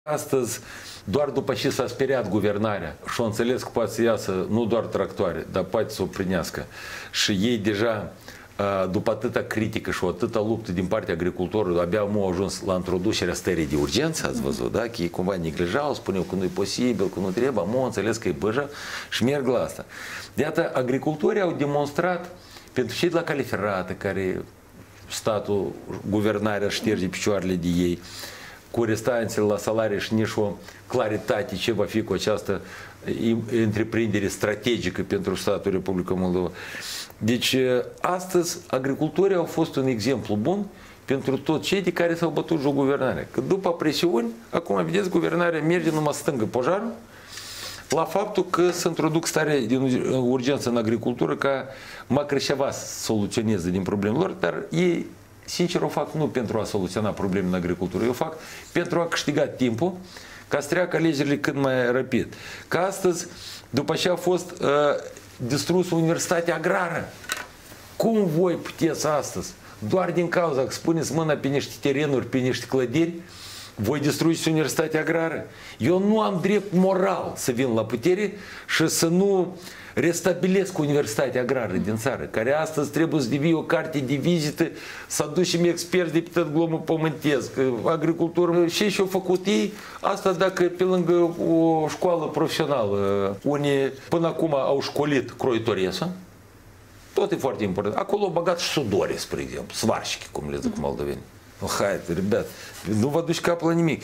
Voi restabili Universitatea Agrară de Stat din Moldova, închisă de guvernarea PAS!
Voi restabili Universitatea Agrară de Stat din Moldova, închisă de guvernarea PAS! Cum puteți să distrugeți Universitatea Agrară doar pentru ca să puneți mâna pe terenuri și clădiri?

Eu nu am dreptul moral să vin la putere și să nu restabilesc Universitatea Agrară, care trebuie să devină o carte de vizită pentru țară. La această universitate trebuie să aducem experți din toată lumea. Ceea ce au făcut ei este ca, de exemplu, într-o școală profesională unde sunt croitorese au integrat și sudorii. Nu îi duce capul la nimic.

Și în cazul fermierilor, guvernarea doar după ce s-a speriat de protestele anunțate de către aceștia, au anunțat că vor introduce Starea de urgență în agricultură. Cei de la conducere vor introduce Starea de urgență nu pentru a soluționa problemele din agricultură, ci pentru a câștiga timp să treacă peste alegerile prezidențiale. Totuțși, fermierii sunt un bun exemplu când luptă pentru drepturile lor. La fel trebuie să facă și angajații la Căile Ferate din Moldova, dar și reprezentanții altor domenii.
Обязуюсь восстановить Молдавский государственный аграрный университет, который закрыла PAS! Как можно было разрушить этот вуз только для того, чтобы захватить землю и здания?!

У меня просто нет морального права прийти к власти и не восстановить Молдавский государсвтенный  аграрный университет! Это высшее учебное заведение должно стать гордостью нашей страны. Мы обязаны привлечь сюда специалистов со всего мира. То, что сделала нынешняя власть, напоминает ситуацию, когда в швейном училище вдруг начинают обучать сварщиков.  Ни на что не способны!

И в случае с фермерами, власти только тогда, когда испугались объявленных протестов, заявили о намерении ввести чрезвычайное положение в сельском хозяйстве. Но они хотят ввести чрезвычайную ситуацию не для решения проблем сельхозпроизводителей, а для того, чтобы выиграть время до президентских выборов. Тем не менее, фермеры являются хорошим примером того, как нужно бороться за свои права. Так же должны поступать и железнодорожники, и представители других отраслей.